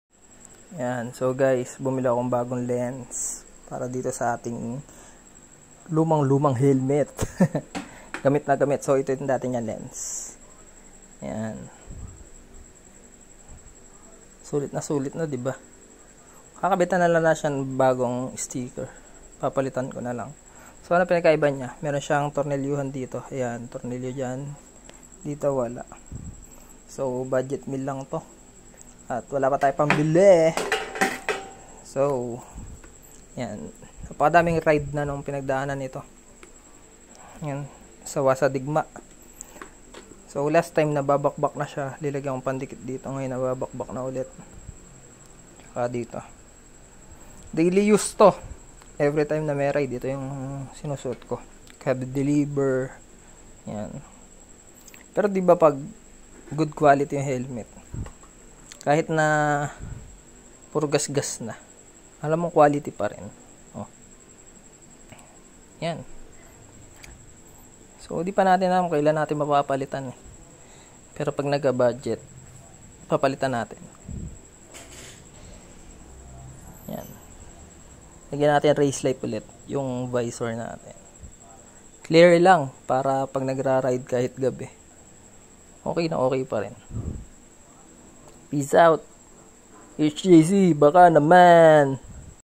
HJC HELMET
Bumili akong murang Visor para sa HJC Helmet ko